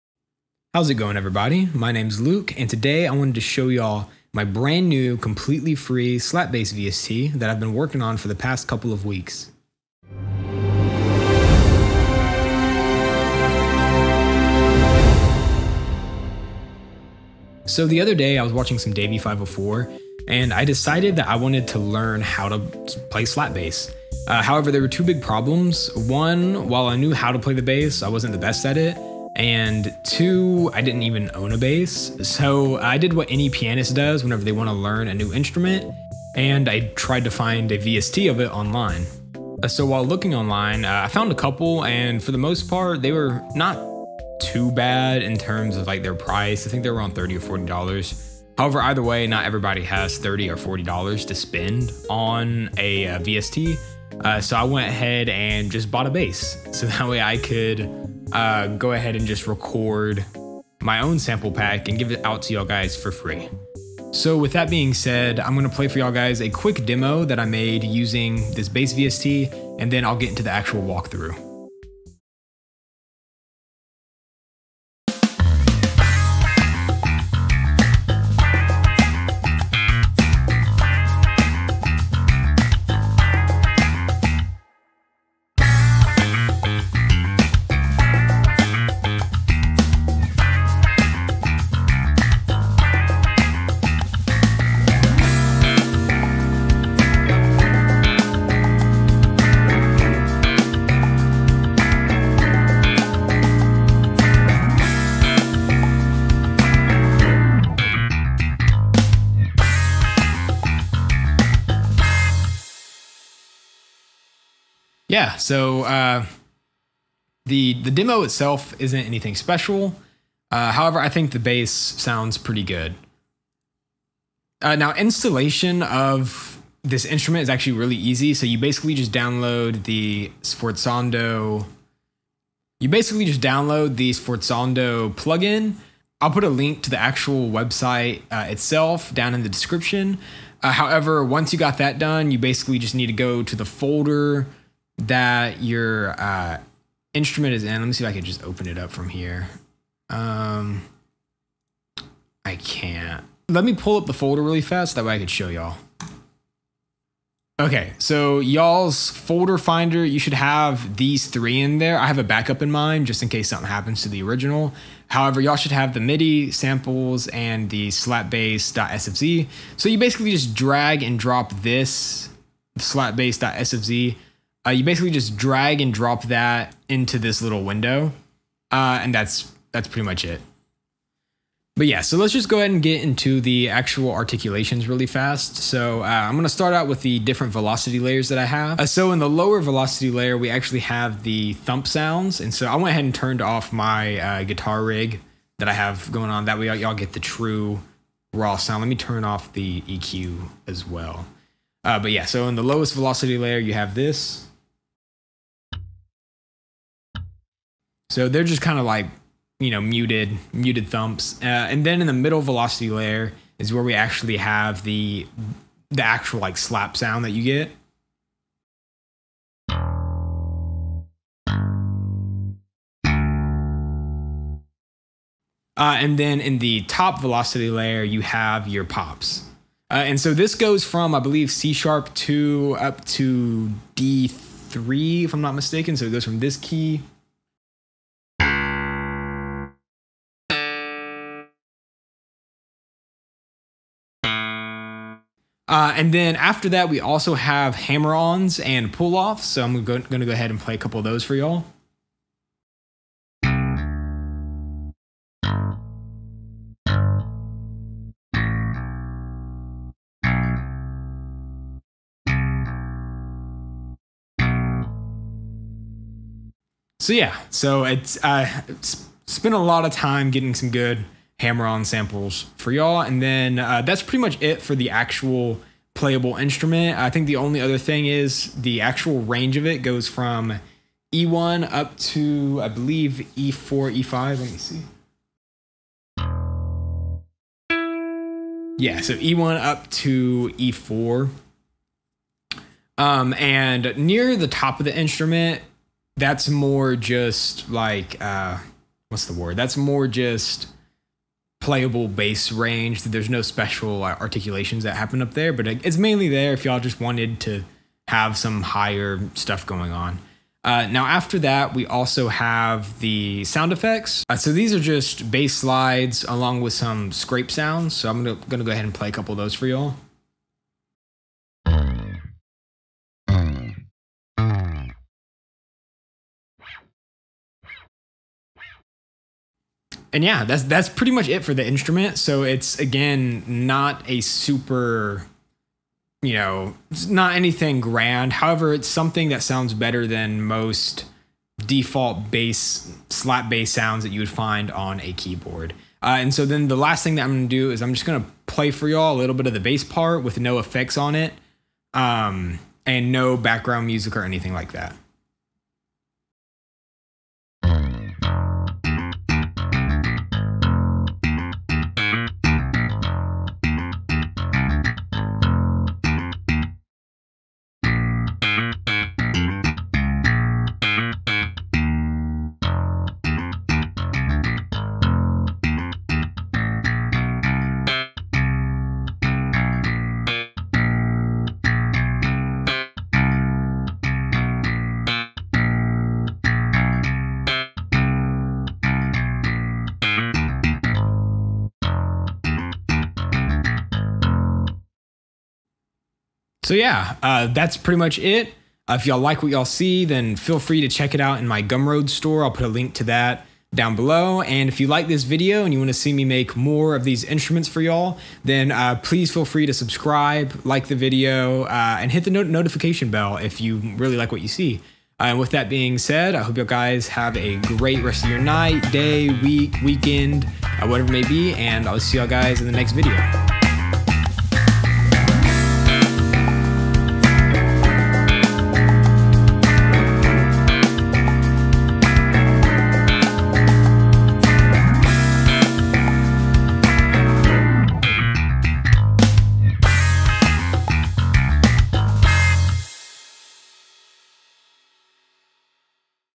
FREE Slap Bass VST 2021 | Realistic SFZ Slap Bass VST
Other Free VSTs used in the demo

Chapters:
0:00 Intro
1:13 Demo
2:03 Installation
3:01 Walkthrough
5:56 Demo (Bass Solo)
6:35 Outro

Thanks for checking out the video and have a blessed day!!!
Feedback welcome! :)